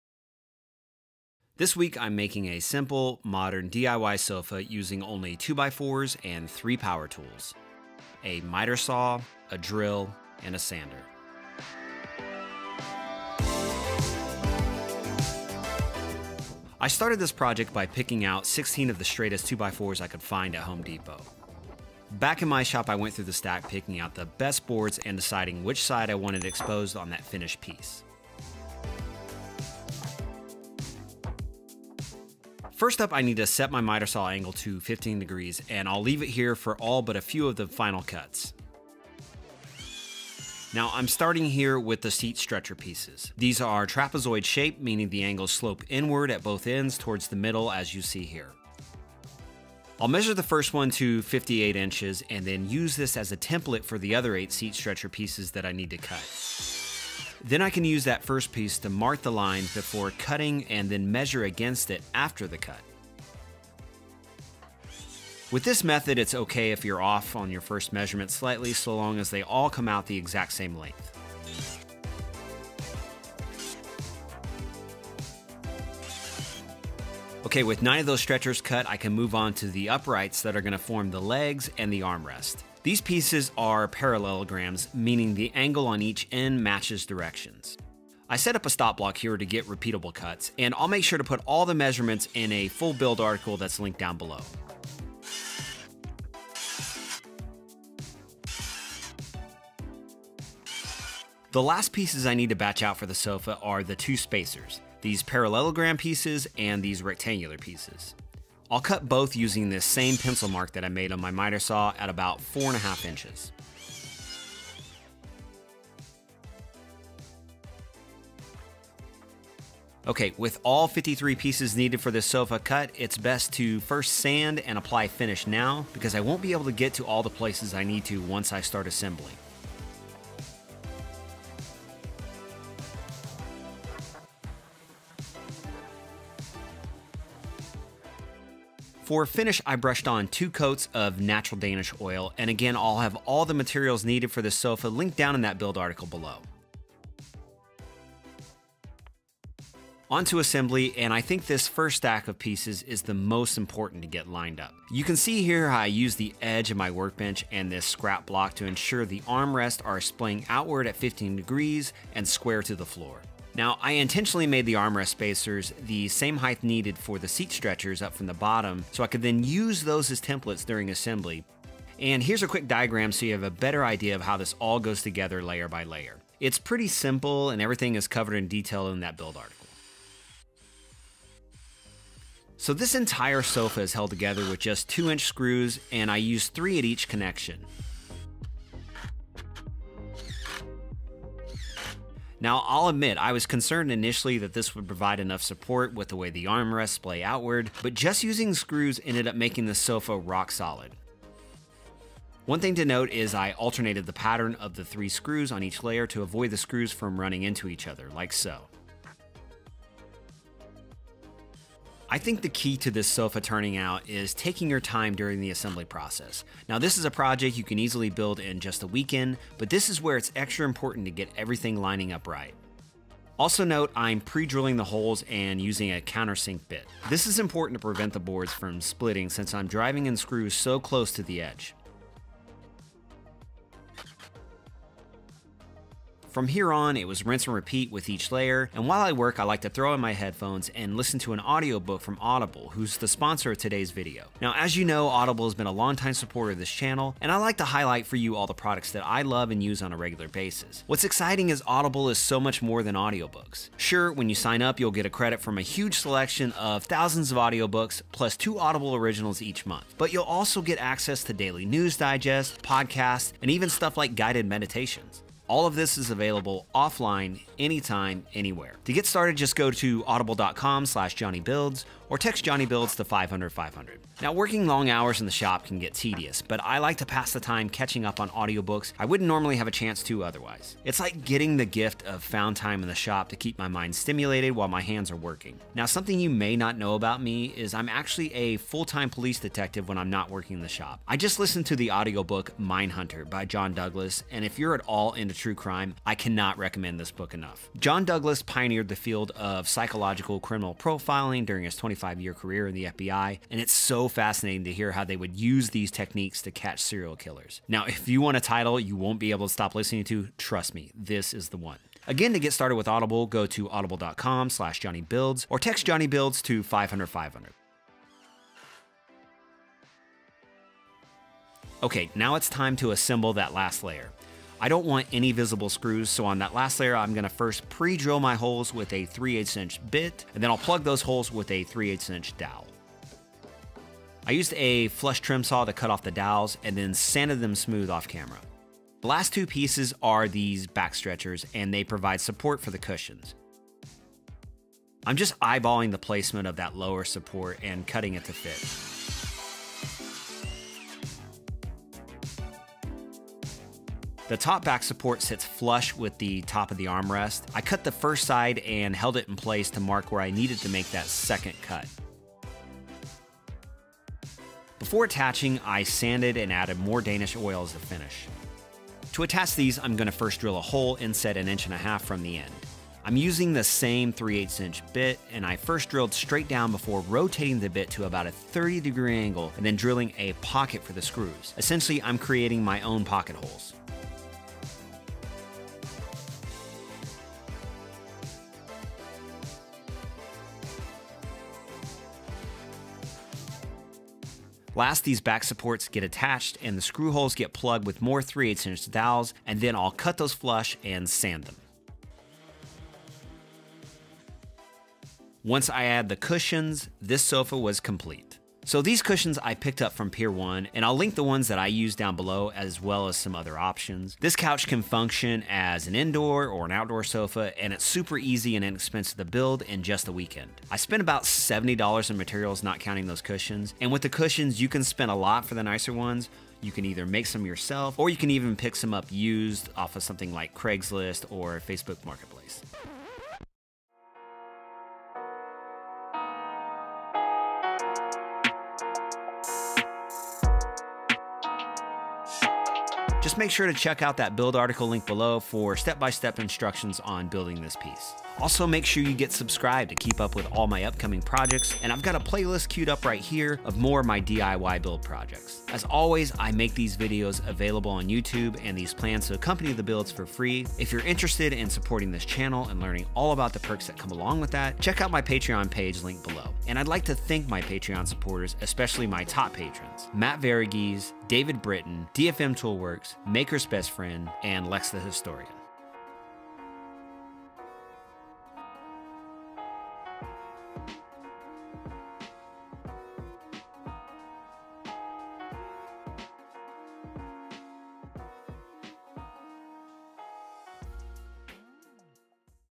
DIY Modern Indoor/Outdoor Sofa Made From 2x4's
This indoor/outdoor DIY modern sofa is made using only 2x4's and 3 tools. Get your first audiobook and two Audible Originals for free when you try Audible for 30 days or text "jonnybuilds" to 500 500!

This indoor or outdoor sofa is made using 3 common tools: a miter saw, a drill, and a sander. The materials are easy to source being that its just 16 2x4 studs. This sofa has a simple modern design, and is probably the easiest DIY project I've created yet. You could easily build this in a weekend.

Materials:

(16) 8 foot long 2x4’s
(1) 3/8th inch pine dowel

Tools:
Miter Saw

Jesse Tubbs